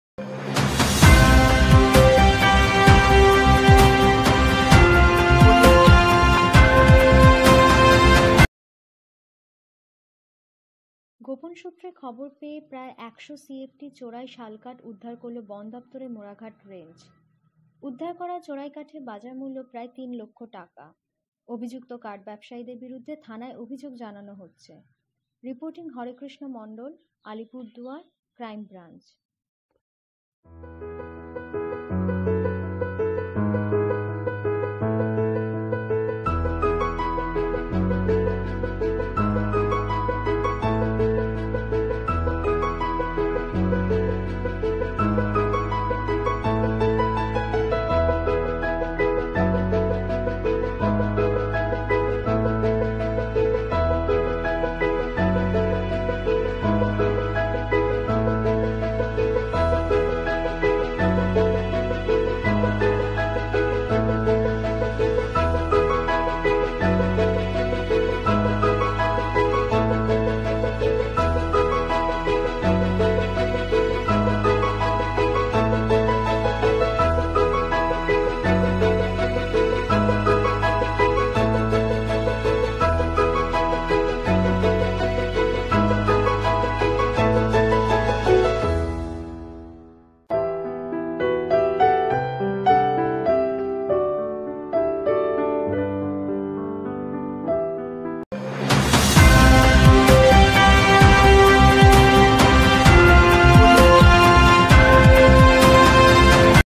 গোপন সূত্রে খবর পেয়ে প্রায় ১০০ সিএফটি শাল কাঠ উদ্ধার করল বনদপ্তরের মোরাঘাট রেঞ্জ
হরেকৃষ্ণ মন্ডল। আলিপুর দুয়ার। ক্রাইম ব্রাঞ্চ।

গোপন সুত্রে খবর পেয়ে প্রায় একশো সি এফ টি চোরাই শাল কাঠ উদ্ধার করল বনদপ্তরের মোরাঘাট রেঞ্জ। উদ্ধার করা চোরাই কাঠের বাজার মূল্য প্রায় ৩ লক্ষ টাকা। রবিবার দুপুরে গোপন সূত্রে খবর পেয়ে স্টেট আর্মড পুলিশ ফোর্সের কর্মীদের সাথে নিয়ে শালবাড়ি -১ গ্রাম পঞ্চায়েতের চানাডিপা এলাকায় ৩ জন কাঠ ব্যবসায়ী লতিফ মিয়া, গোলাপ হক এবং মহম্মদ হবির বাড়িতে হানা দিয়ে এই কাঠ উদ্ধার করেন বনকর্মীরা। ওই তিন কাঠ ব্যবসায়ীর বিরুদ্ধে বানারহাট থানায় এফ আই আর করতে চলেছে মোরাঘাট রেঞ্জ। মোরাঘাট রেঞ্জের রেঞ্জ অফিসার রাজ কুমার পাল জানান, কাঠের চোরাচালানের বিরুদ্ধে বনদপ্তরের অভিজান জারি থাকবে। অভিজুক্ত কাঠ ব্যবসায়ীদের বিরুদ্ধে থানায় অভিযোগ জানানো হচ্ছে।